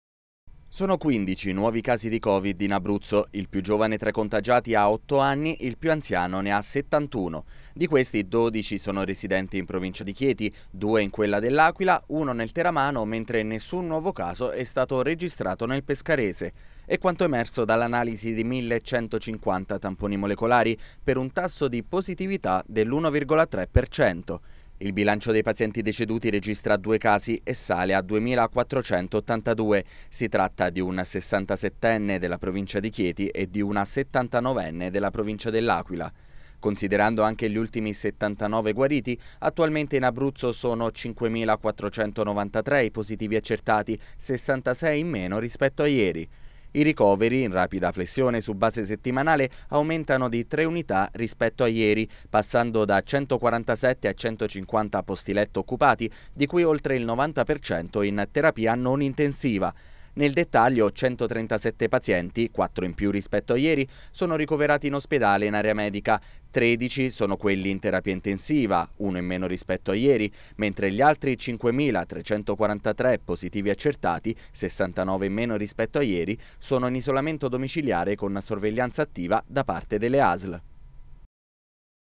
Covid Abruzzo. Oggi 15 nuovi casi e due morti
Bollettino Covid: oggi in Abruzzo 15 nuovi casi (1,3%), due morti e 79 guariti. La provincia di Chieti è la più colpita.